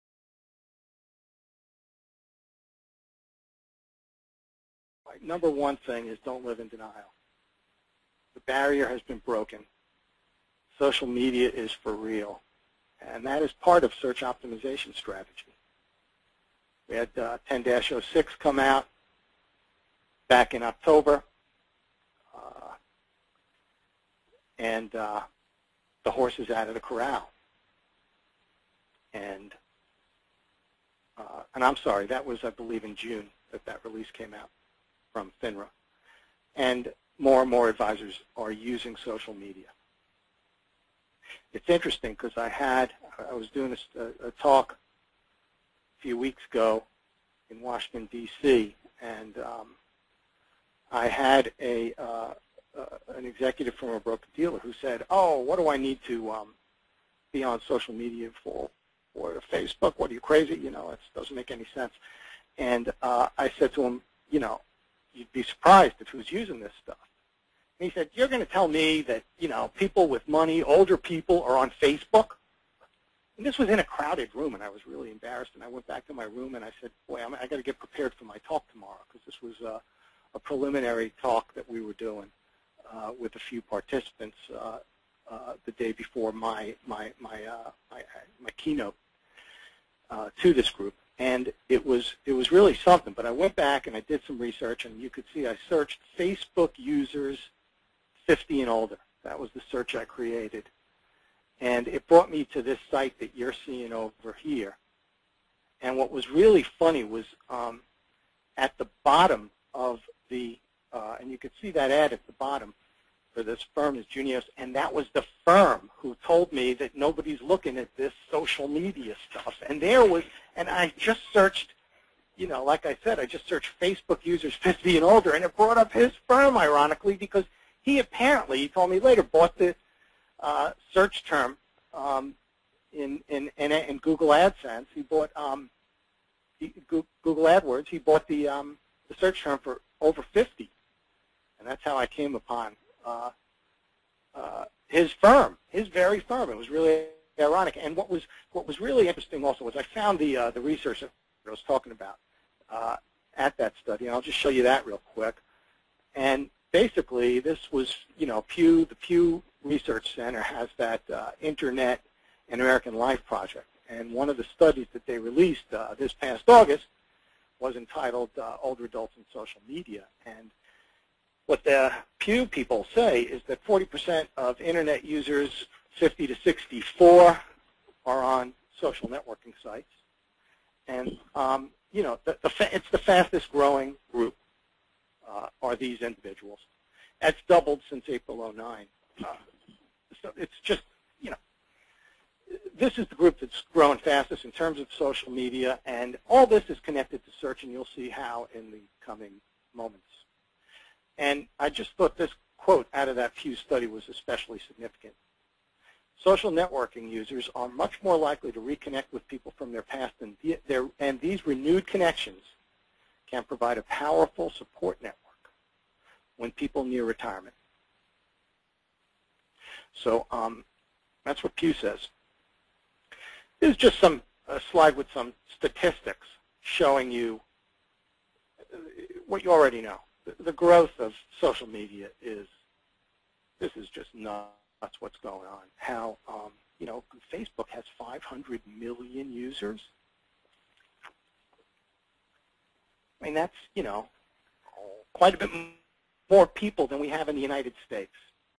Social Media is For Real for Financial Advisors
Social media is important for Financial Advisor Marketing.  Older adults are the fastest growing group of social media users.